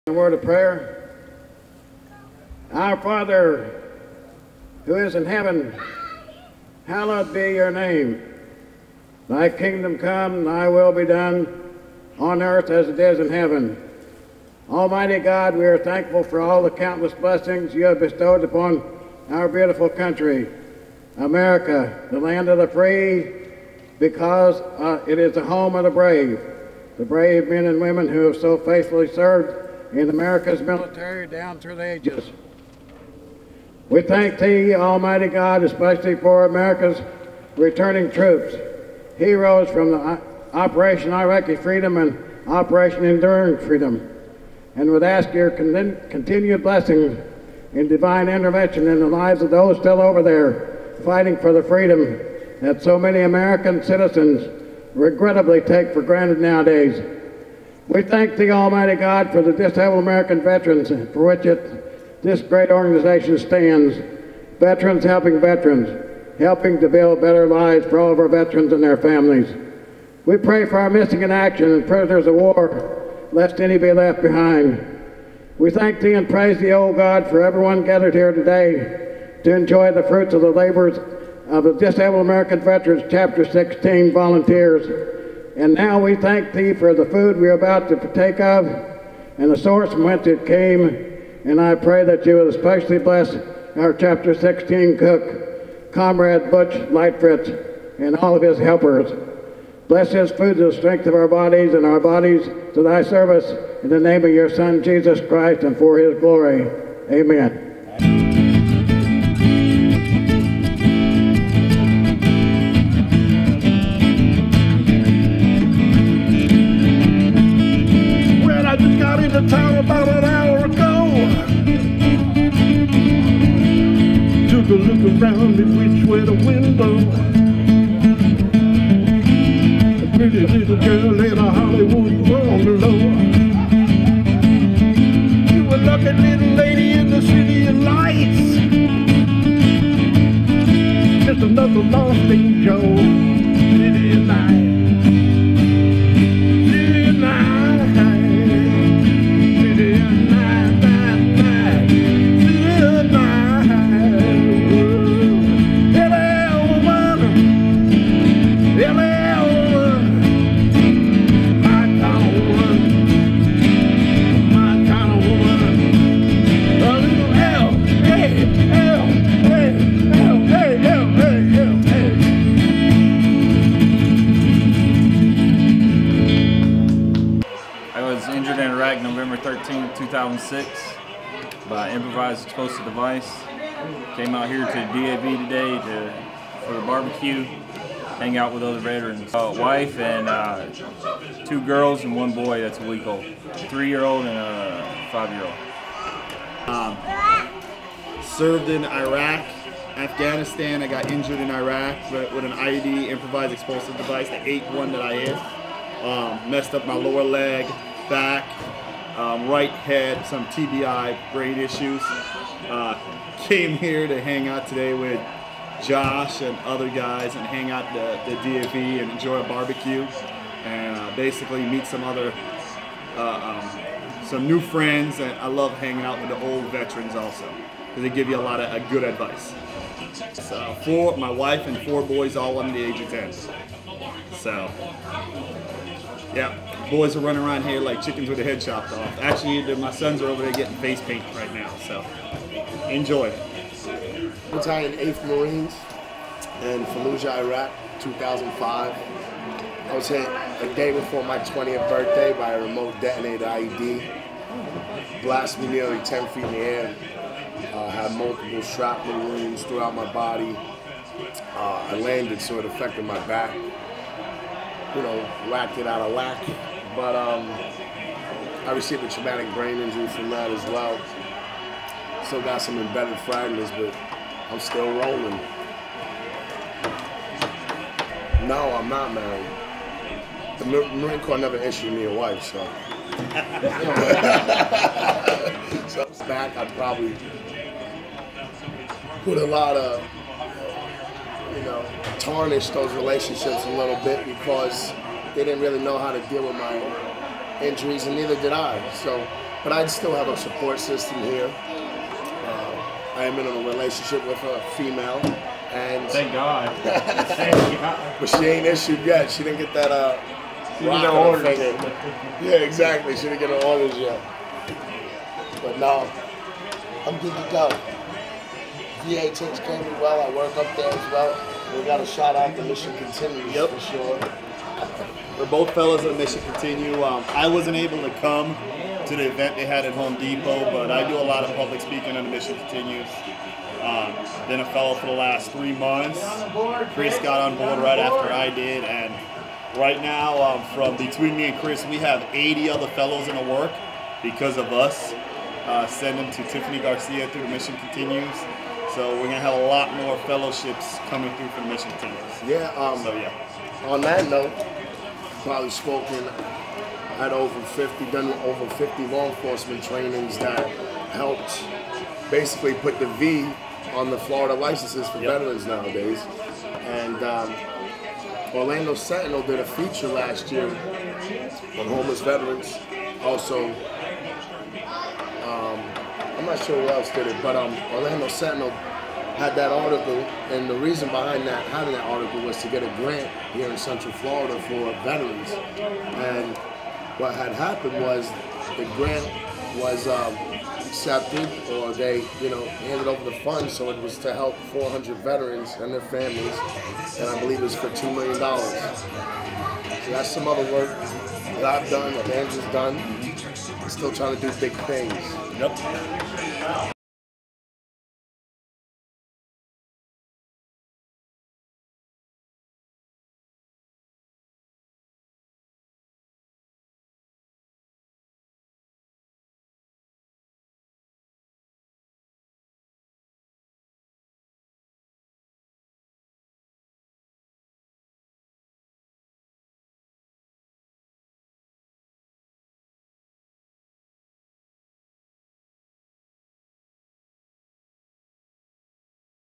Orlando DAV OEF OIF BBQ
October 2, 2011 the Orlando DAV gave the OEF OIF BBQ.  Joshua Cope, Andrew Berry and Christopher O'Connor, spoke about their service and being wounded by IEDs in Iraq.  Big smiles on all three.